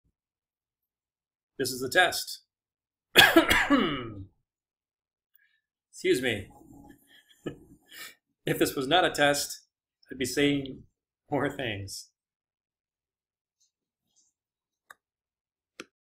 Recording #35
test of equipment prior to the 2016 submersion experiment.  unfortunately the laptop went to sleep in the actual recording and the audio cut out.